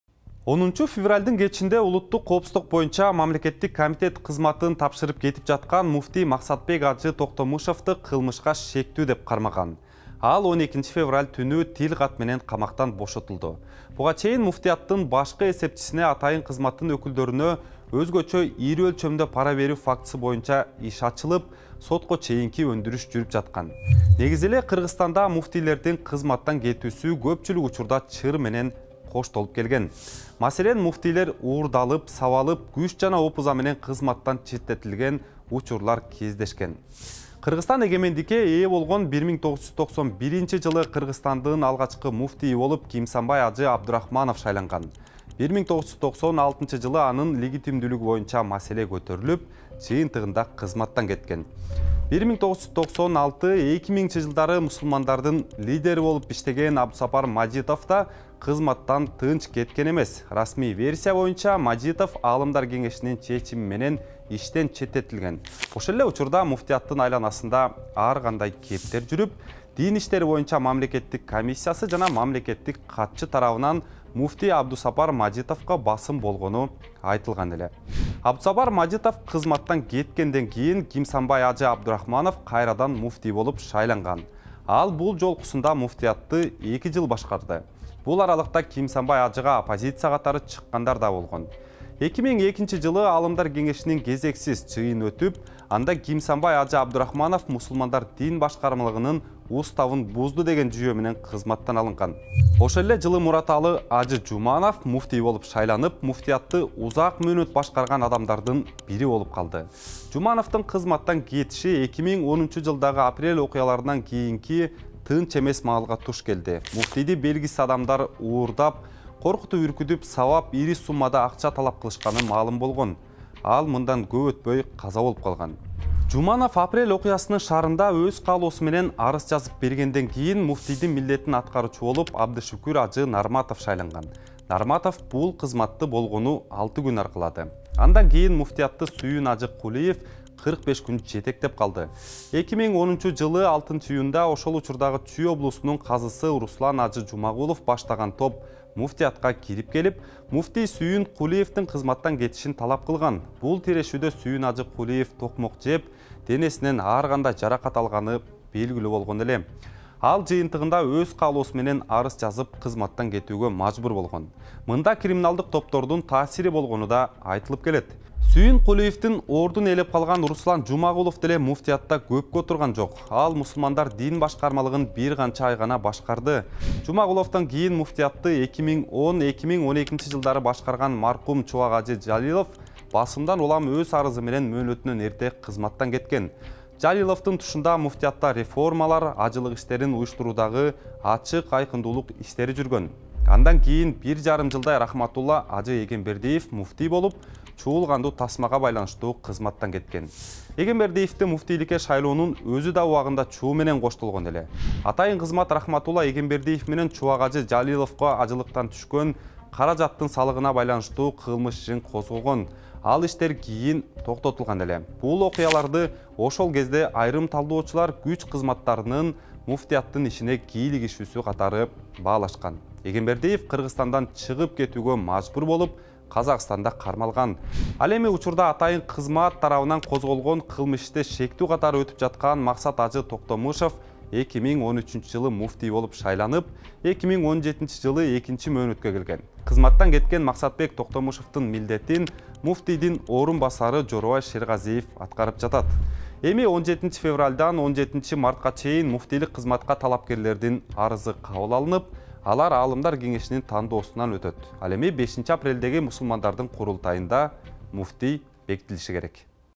Муфтийлер кызматтан кантип кеткен?
Максатбек Токтомушев чыр менен кызматтан кеткен биринчи муфтий эмес. Буга чейин да мусулмандардын лидерлери ар кандай окуялардан улам алмашып келген. Кээ бир учурларда муфтияттын ишине мамлекеттик органдардын кийлигишкени айтылып келет. “Азаттык” Кыргызстандын муфтийлери кандай жол менен кызматын тапшырганына токтолот.